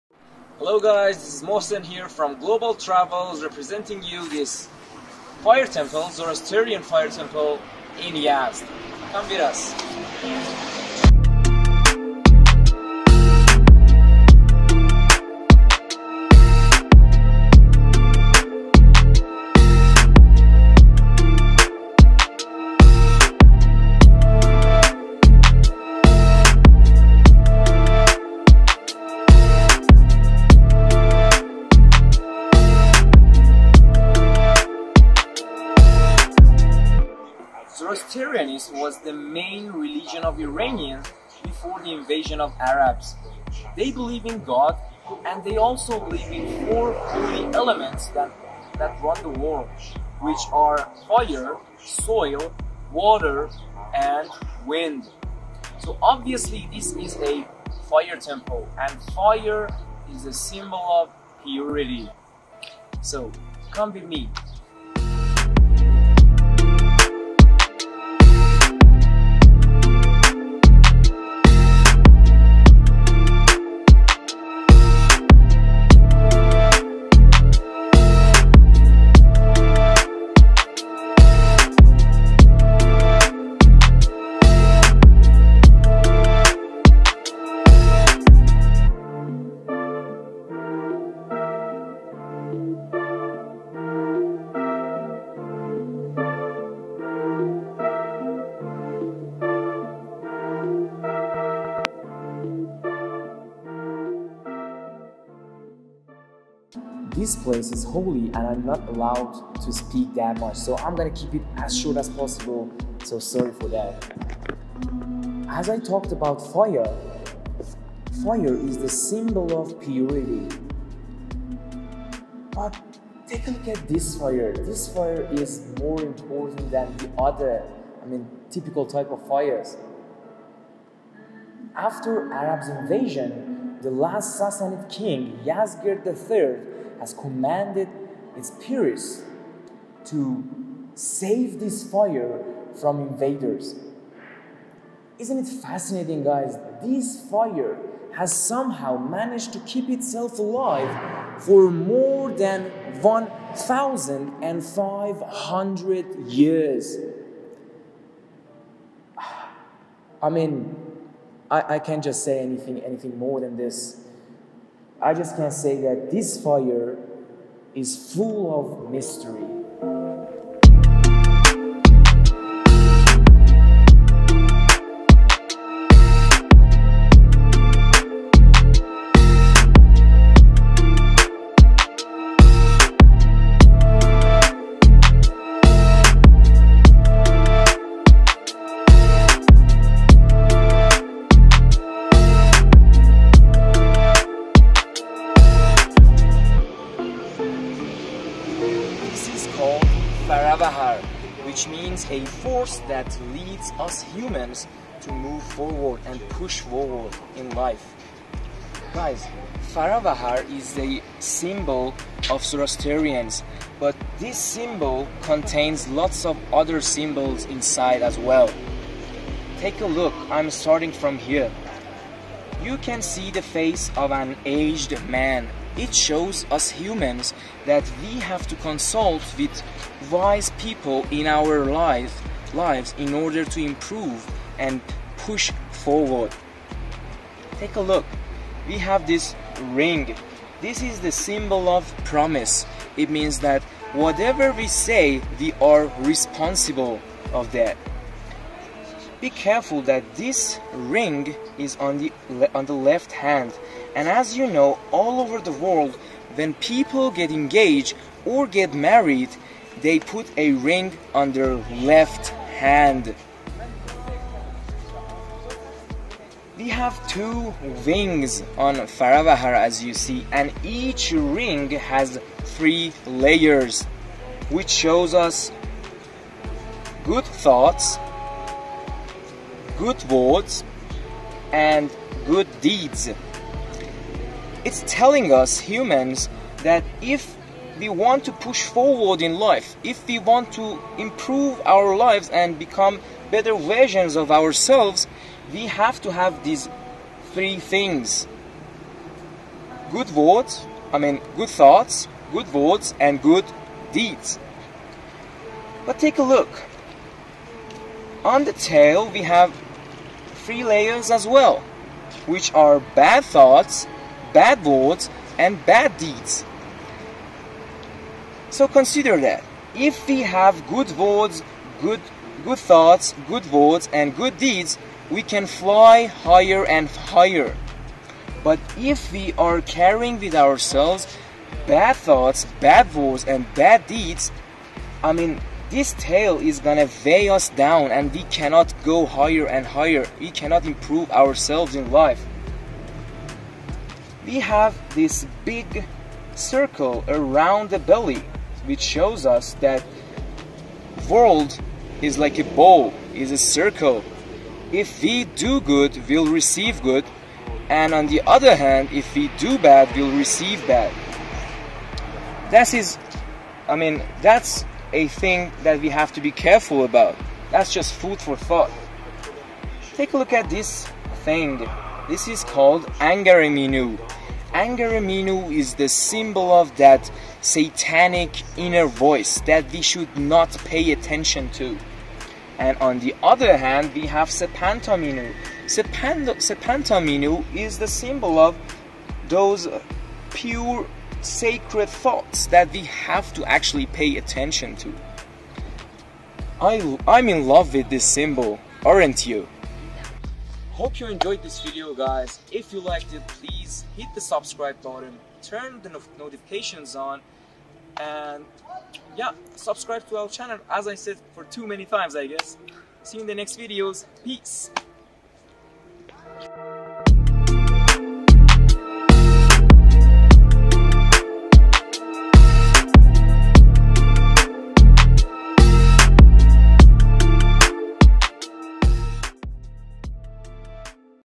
Zoroastrian Fire temple tour in Yazd - [470 AD]
In this video , we show you inside of Yazd fire temple and a fire that has been burning continuously for millennia !
watch the video to find out the mystery behind this fire

you can also check our website for more information regarding Iran